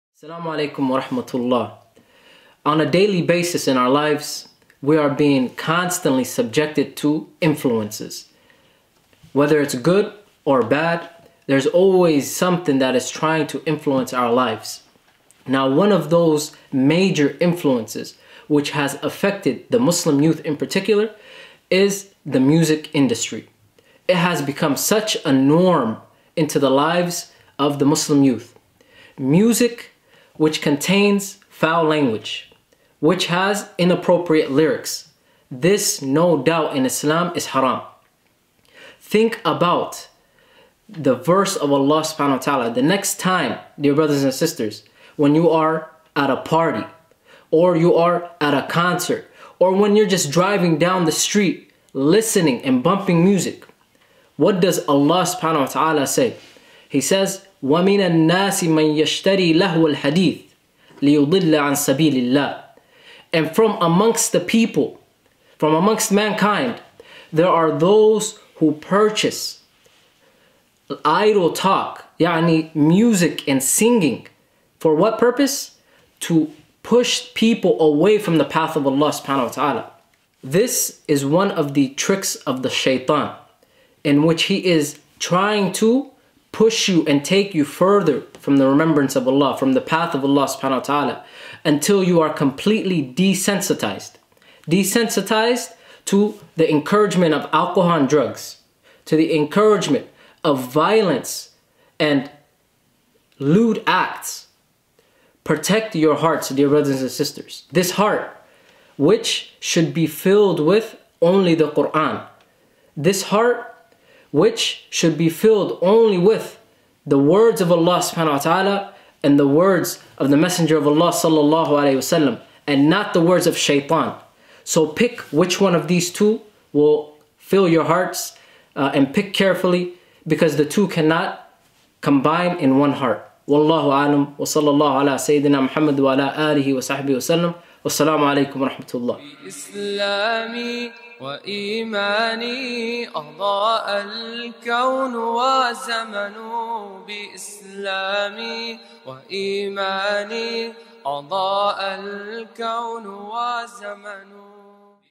THE INFLUENCE OF MUSIC
Music is another phenomena that has been running rampant in the Muslim community lately- which almost all, if not all- encourages and promotes vice/sin. It is the door to the major illnesses of society: drugs, violence, sexual promiscuity, and arrogance. The cure & solution for all of these illnesses is the Qur'an. And the more one listens to the words of the Creator, the more it purifies the heart until it becomes inconceivable to draw near to the instruments of Shaytān. May Allah save us all and purify our hearts and protect our hearts.

CONNECT WITH DEENLOVE